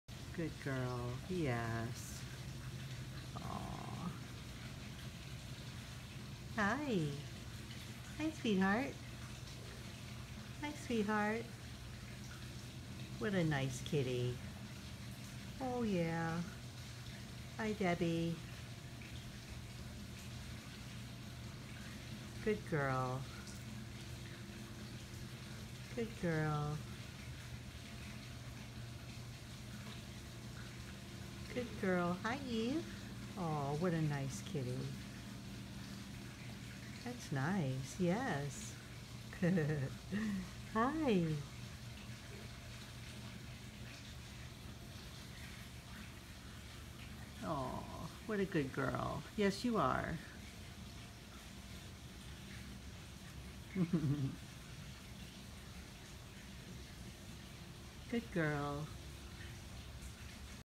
Stray cat, Debbie, meets Eve, the "welcome wagon" cat
I have been attempting to become friends with a stray cat for the last 9 months.  My plan is to take her to the vet for testing and vaccines.  However, she is not allowing  me to pet her.  So... I brought out our indoor cat Eve, aka the welcome wagon, to assist in facilitating a friendship.